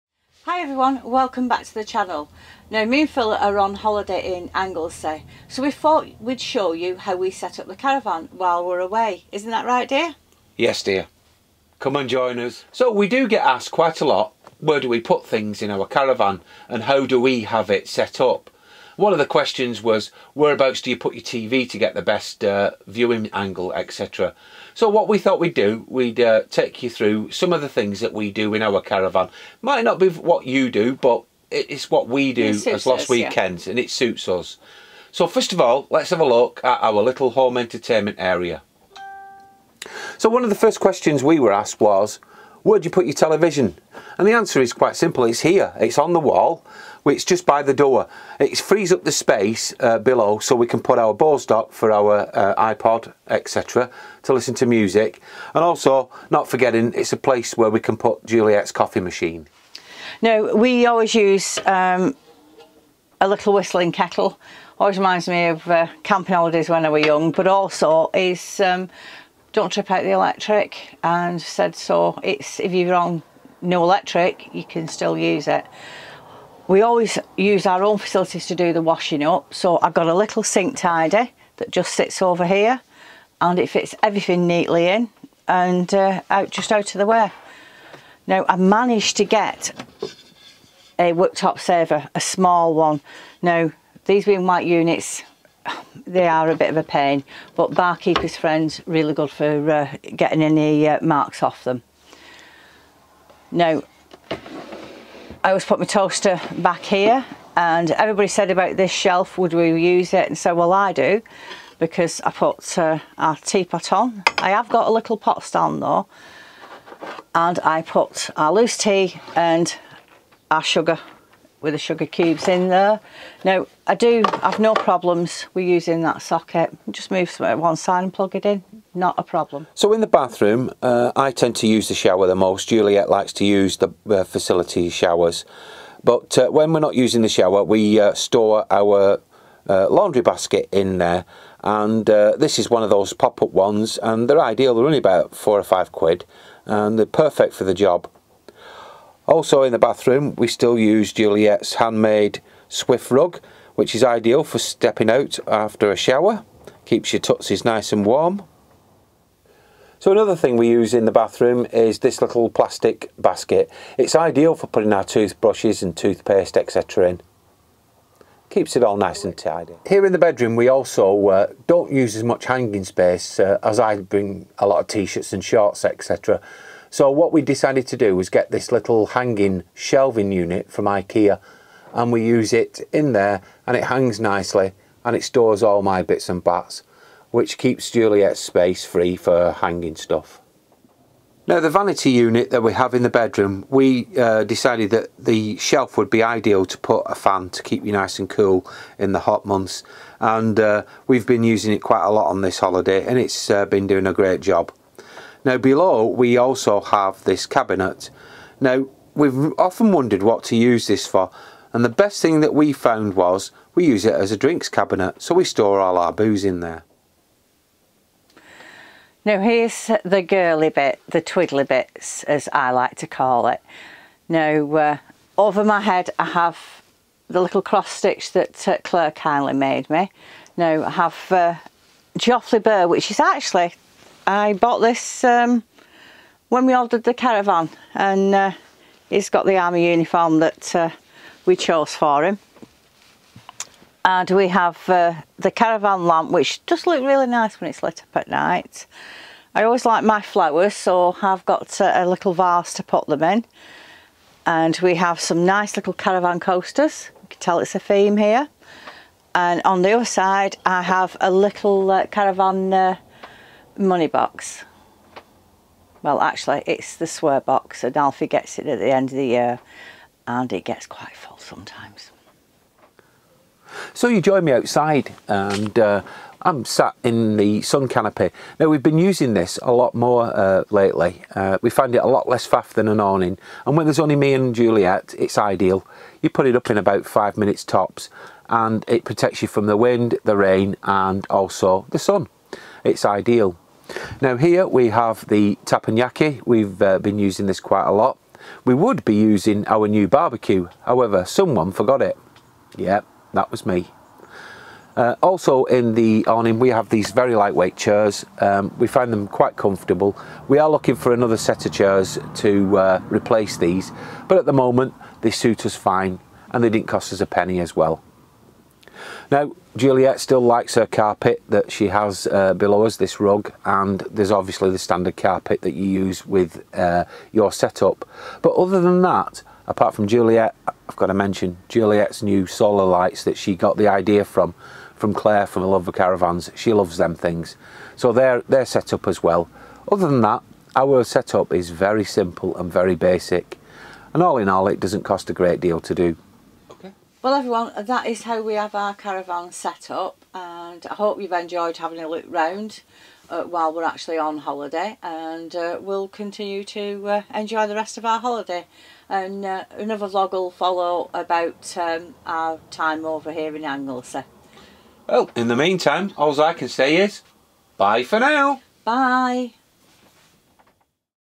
Our Caravan Set-up when on Holiday
Hello Folks. In todays video we show you our Caravan Set whilst away. It is a lot simpler than you may suspect. Phil

Todays was not a promo video but some of you lovely folk have been interested in a few of the things we use so I have put a few links below.

Hanging Shelf Best Price.
Flame Outdoor LED Lights.